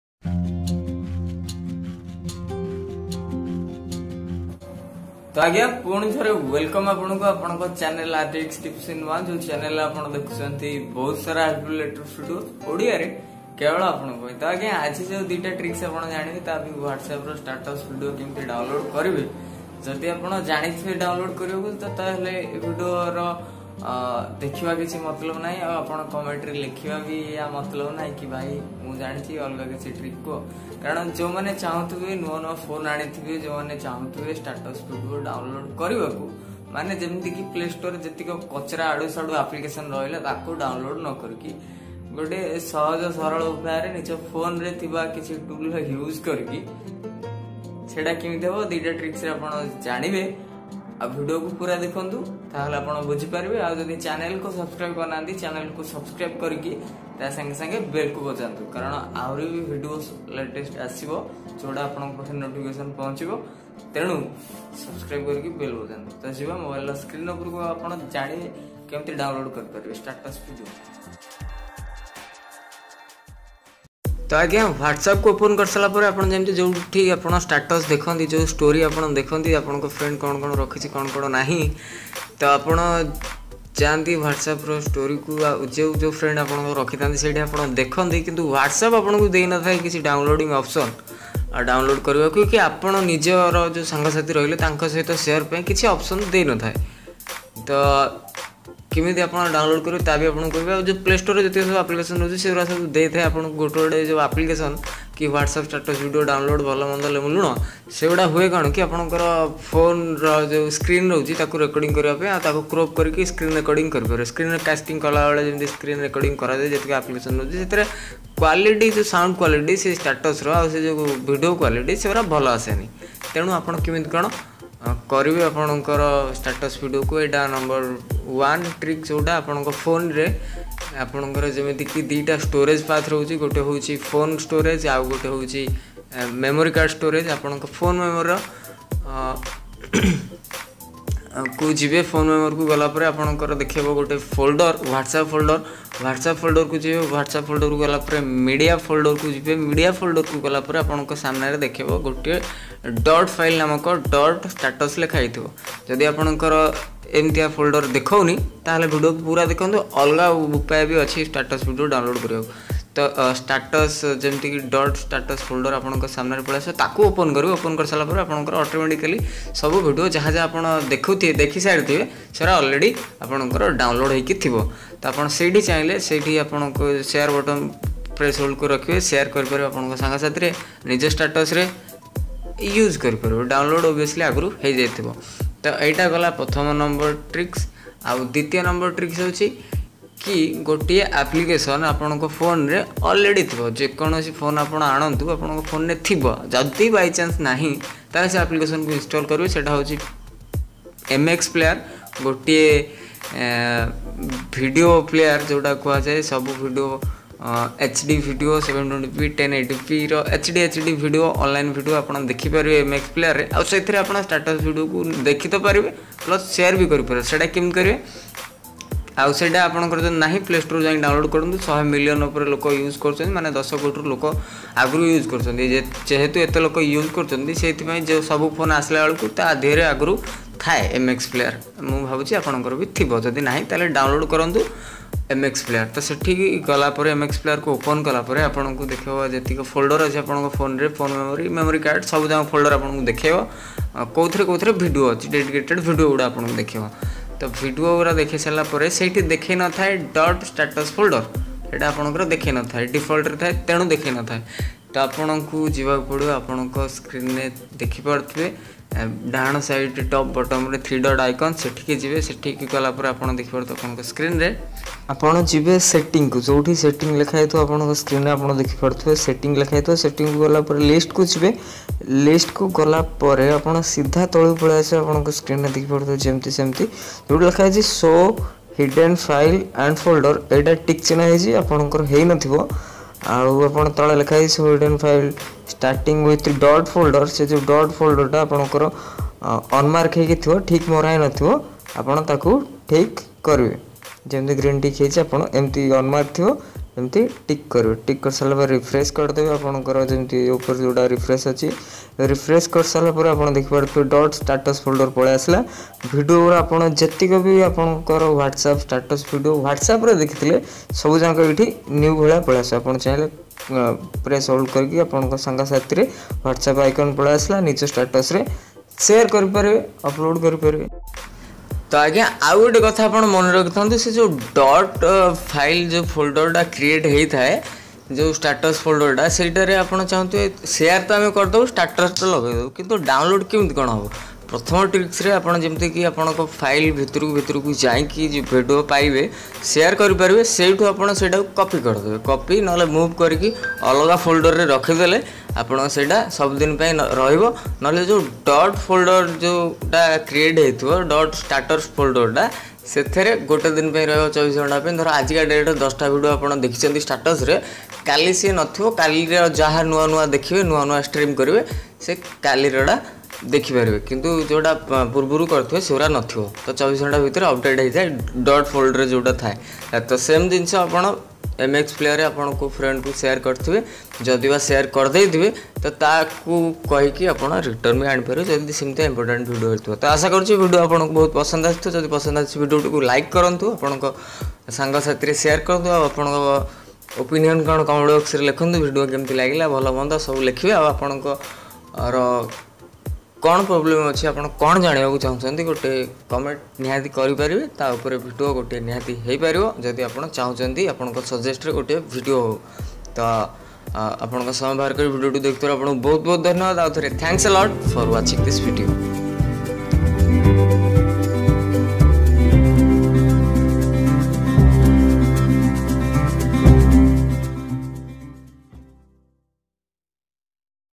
2 Best Tricks to Download WhatsApp Status Videos Any Smartphone By: Rdx Tips in One
2 Best Way to Download WhatsApp Status Videos on Smartphone without using any Third-party
# Application
ଓଡ଼ିଆ ରେ
as your own responsibility.
If you are interested and agree !!
Help to  other  people ..